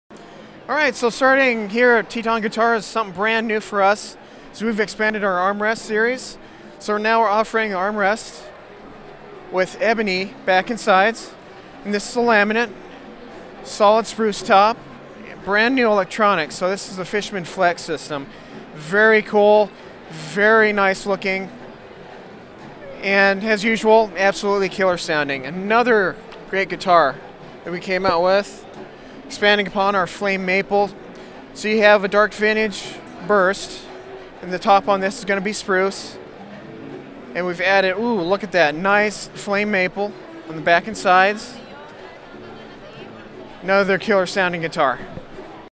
Winter NAMM 2019 - Teton Guitars
Mark from Teton Guitars shows two of the company's newest offerings at the 2019 Winter NAMM show.